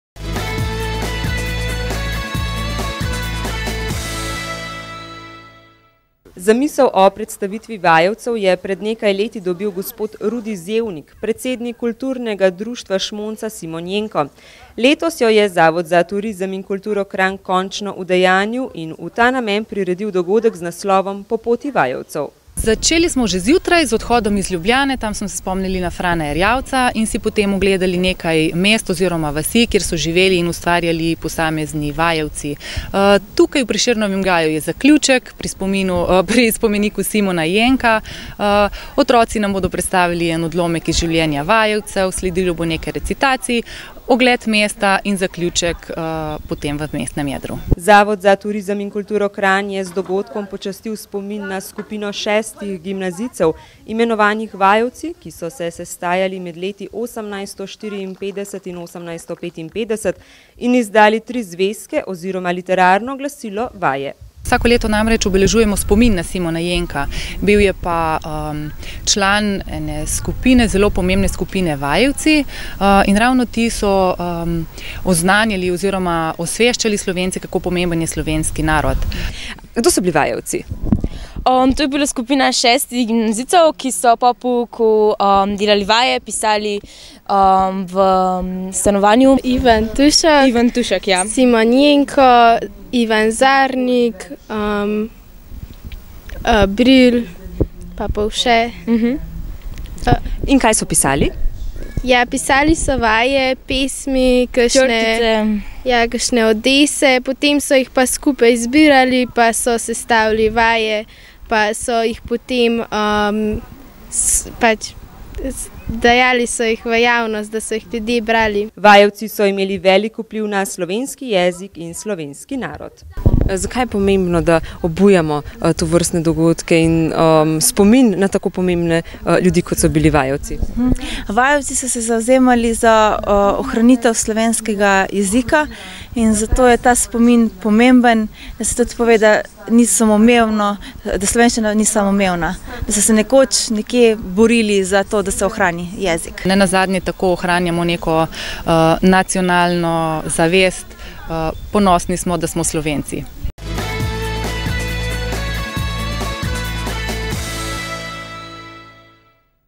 GORENJSKA POROČILA: Po poti vajevcev
Zavod za turizem in kulturo Kranj je pretekli teden v Prešernovem gaju priredil zaključni dogodek Po poti Vajevcev. Dogodek je bil posvečen Vajevcem, ki so delovali na Gorenjskem. V ta namen je potekal tudi celodnevni izlet, po katerem so v Prešernovem gaju nastopili učenci Osnovne šole Simona Jenka iz Kranj, ki so odigrali 15min prizor srečanja Vajevcev. Sledile so recitacije avtorskih del gimnazijcev iz Gimnazije Kranj in Gimnazije Franceta Prešerna.